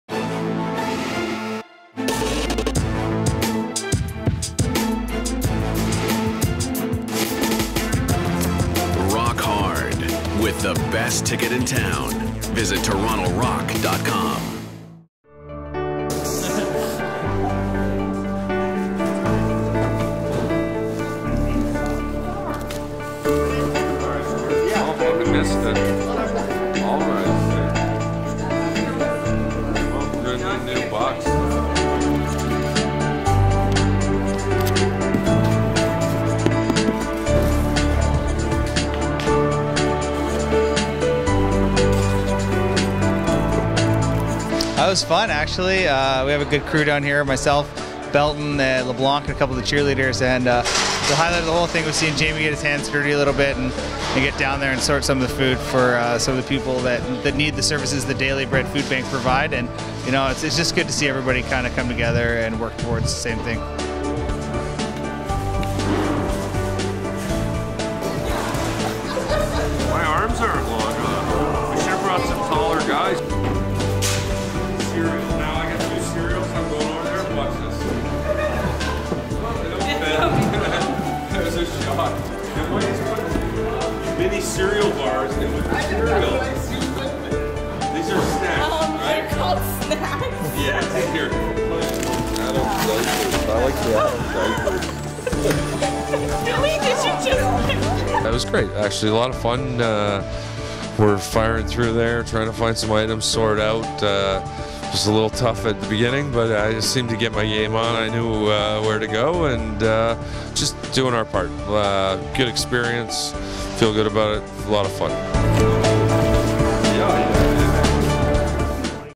Toronto Rock Visit Toronto's Daily Bread Food Bank
Rock players Kyle Belton, Stephan Leblanc and Garrett Billings along with Rock Owner Jamie Dawick and members of Rock Cheer visited Toronto's Daily Bread Food Bank ahead of Friday's Charity Night game against Colorado.